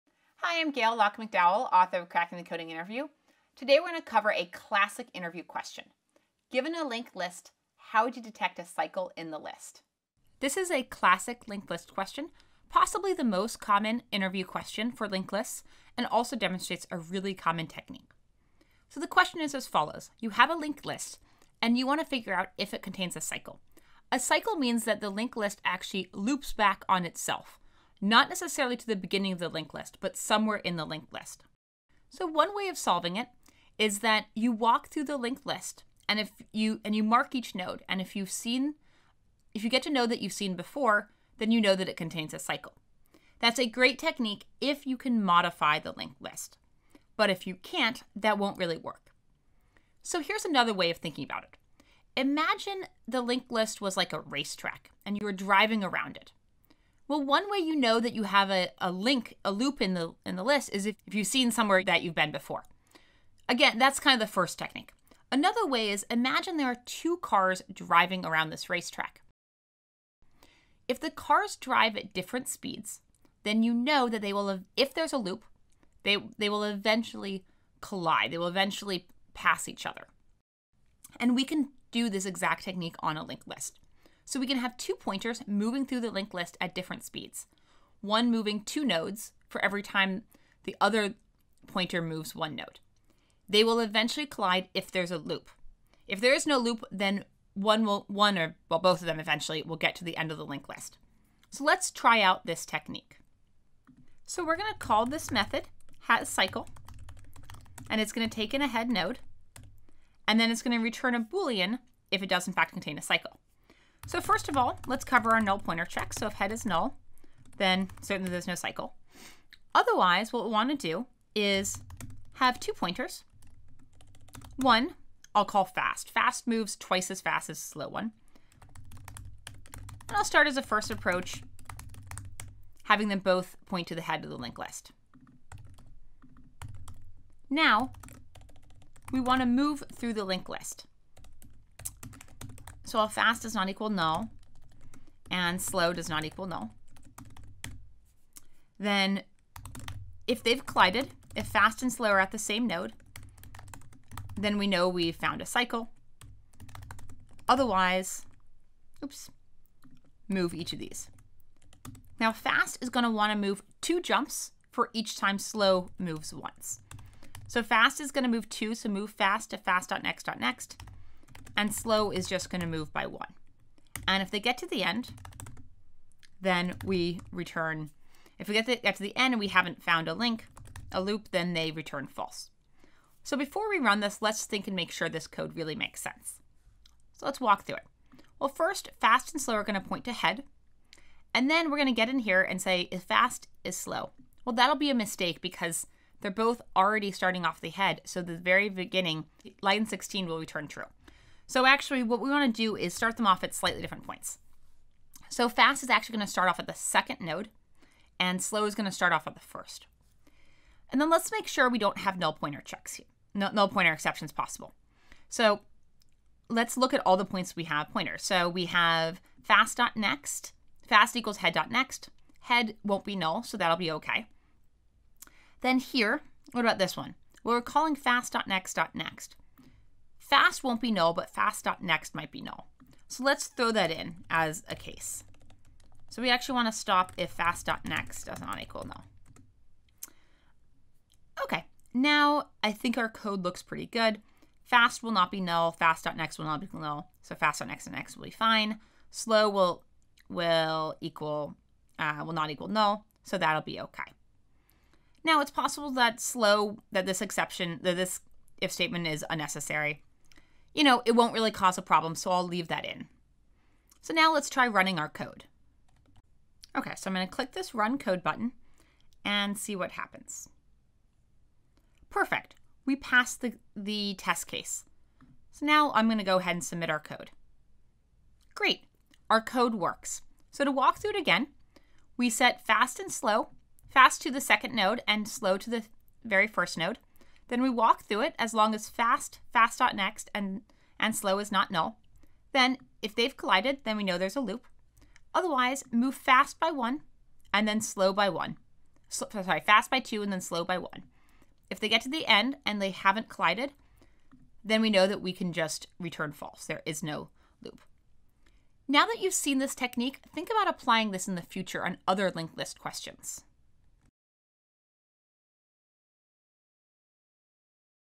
Data Structures: Cycles in a Linked List
Learn how to solve the most common interview question for Linked Lists. This video is a part of HackerRank's Cracking The Coding Interview Tutorial with Gayle Laakmann McDowell.